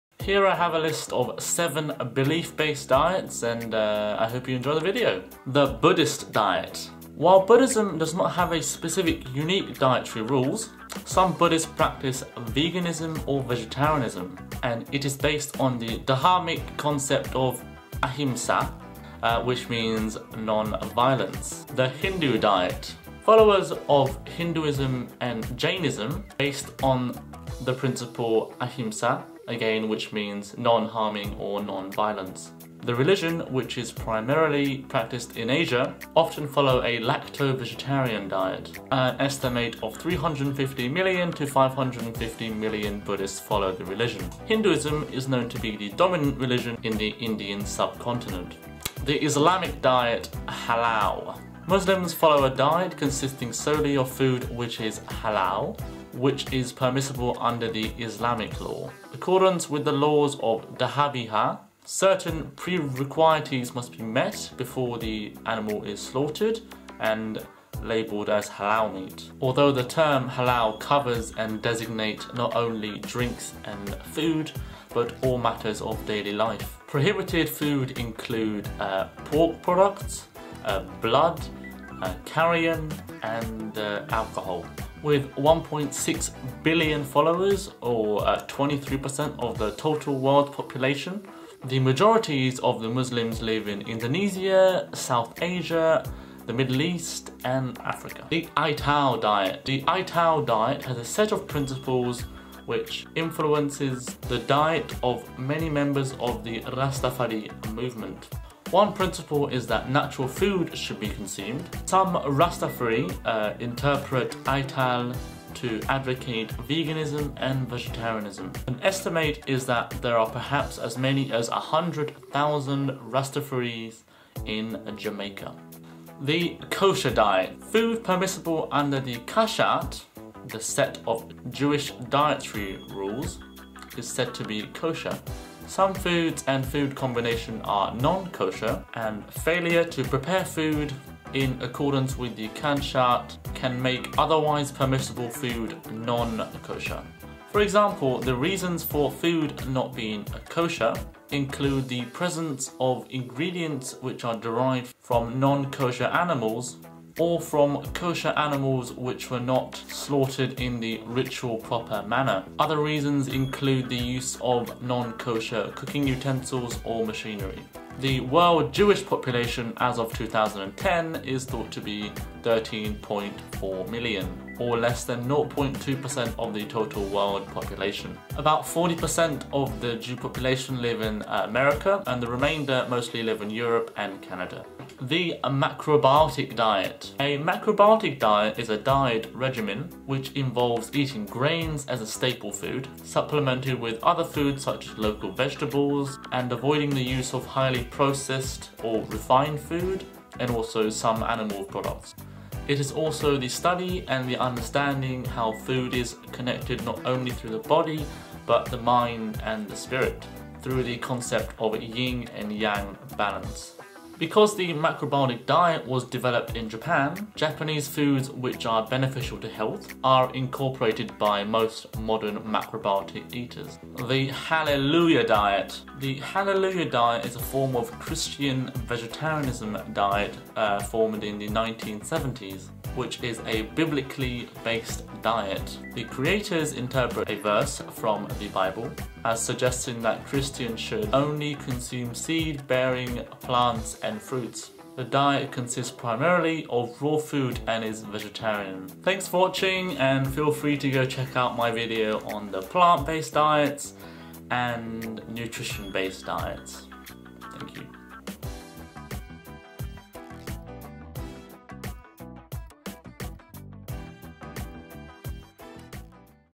Belief Based Diets (Halal, Kosher, Macrobiotic, Hindu, Buddhist etc)
This is the list of popular belief based diets.